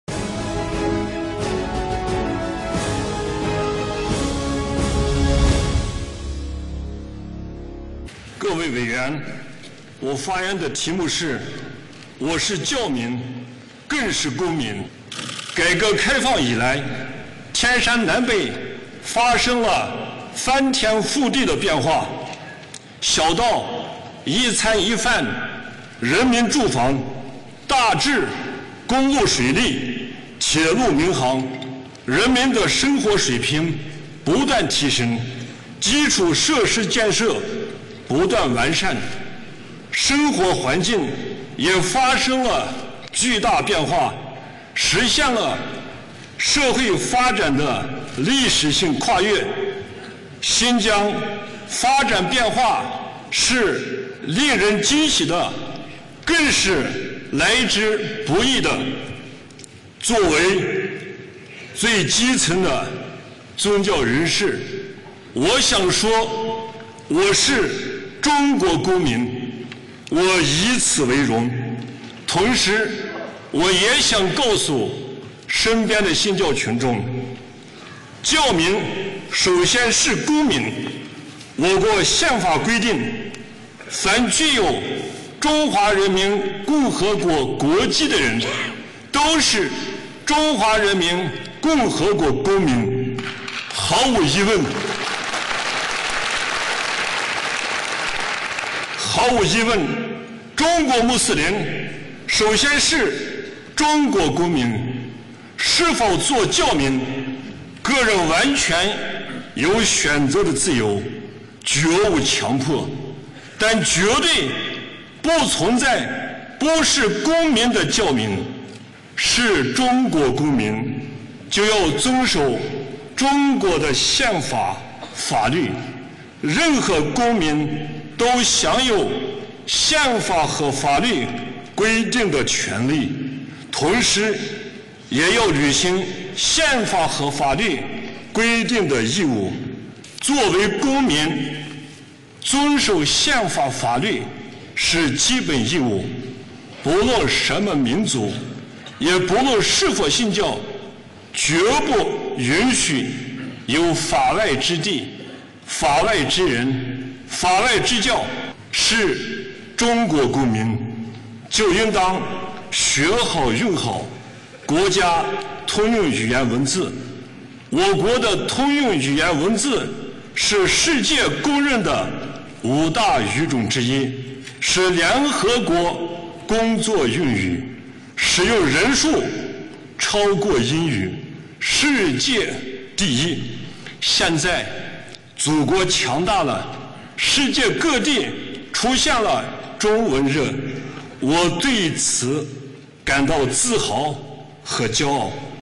杨杰：我是教民更是公民
3月11日，全国政协十三届二次会议举行第四次全体会议，新疆维吾尔自治区呼图壁县政协副主席、伊斯兰教协会会长，呼图壁县城镇陕西寺阿訇杨杰作了题为《我是教民更是公民》的大会发言。